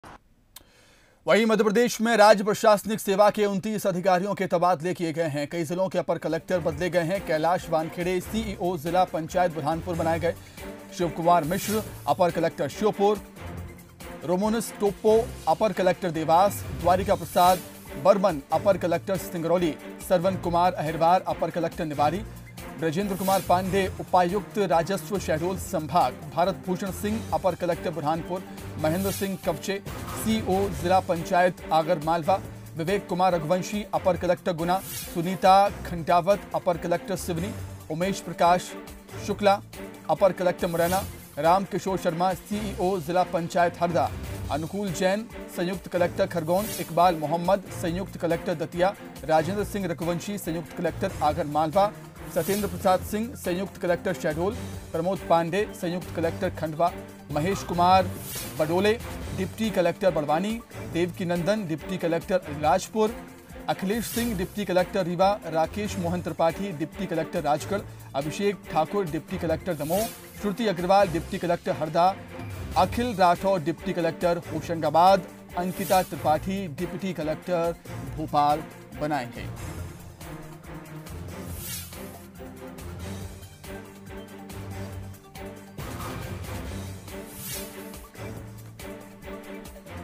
भोपाल : राज्य प्रशासनिक सेवा के 29 अधिकारियों के तबादले, कई जिलों के अपर कलेक्टर बदले गए
कैलाश वानखेड़े- CEO, जिला पंचायत, बुरहानपुर
शिवकुमार मिश्र- अपर कलेक्टर, श्योपुर
रोमोनुस टोप्पो- अपर कलेक्टर, देवास